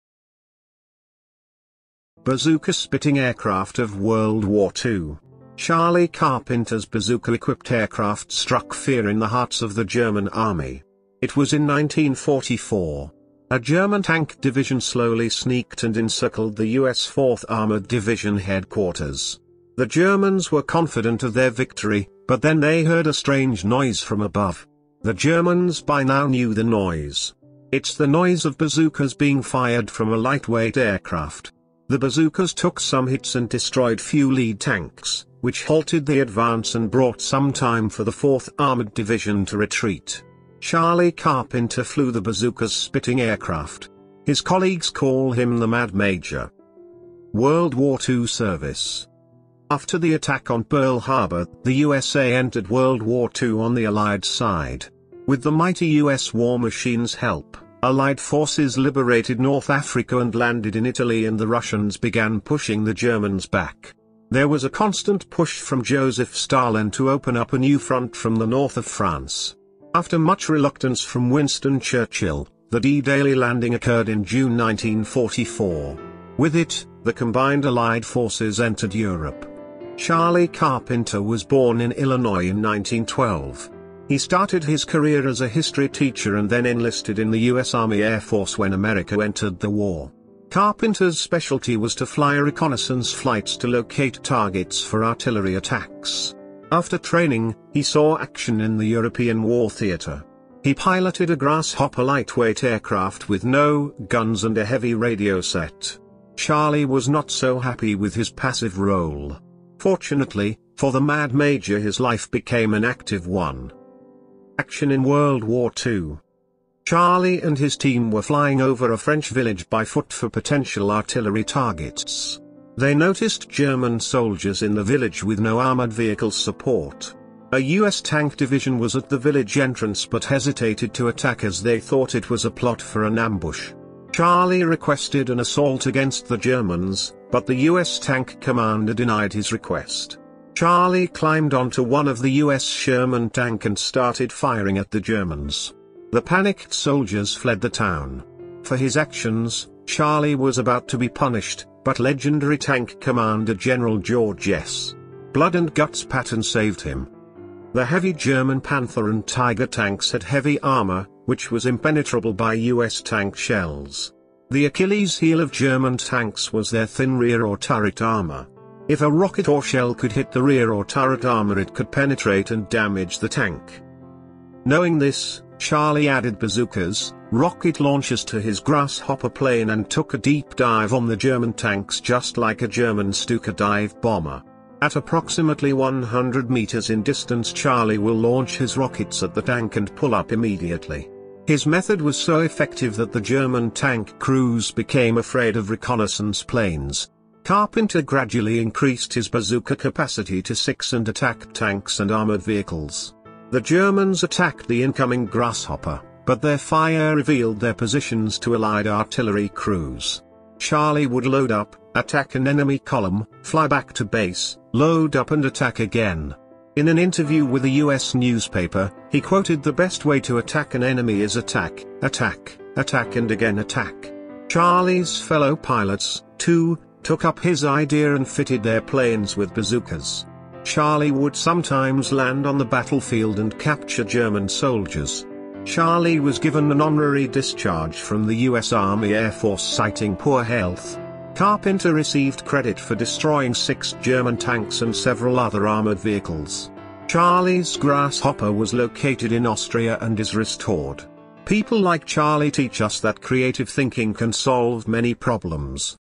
Bazooka Spitting Aircraft of World War II
Charlie Carpenter’s bazooka-equipped aircraft struck fear in the hearts of the German Army.
It was in 1944. A German tank division slowly sneaked and encircled the US 4th armored division headquarters.
The Germans were confident of their victory, but then they heard a strange noise from above. The Germans by now knew the noise.
It’s the noise of bazookas being fired from a lightweight aircraft. The bazookas took some hits and destroyed few lead tanks, which halted the advance and brought some time for the 4th armored division to retreat.
Charlie Carpenter flew the bazookas spitting aircraft. His colleagues call him the “Mad Major.”